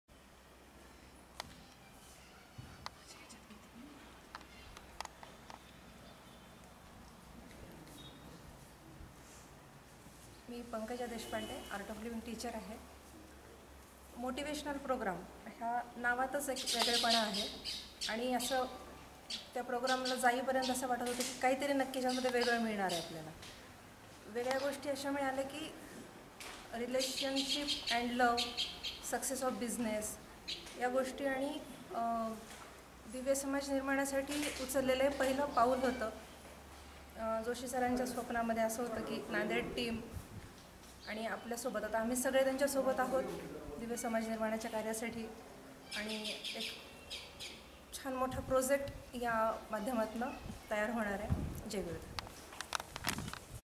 Pankaja Deshpande
Art of Living Teacher, Nanded
Participated in First Motivational Program
(Self Improvement in the Art of Living)
24th to 29th July 2018